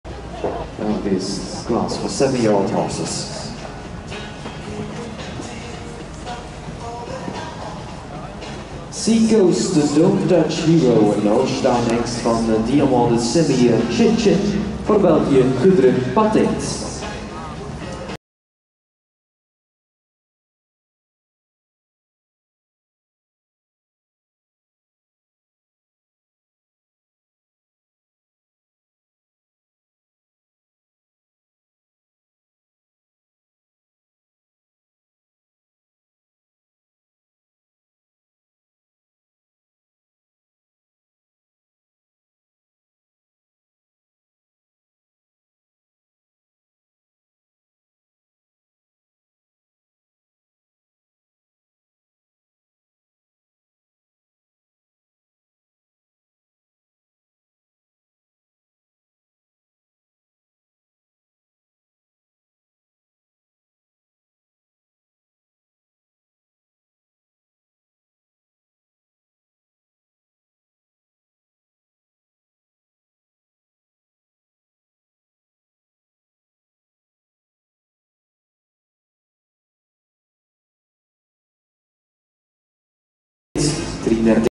Gudrun Patteet & Sea Coast Don't touch tiji hero 7j CSI3* Beervelde
7yo stallion by Diamant de Semilly & Chin Chin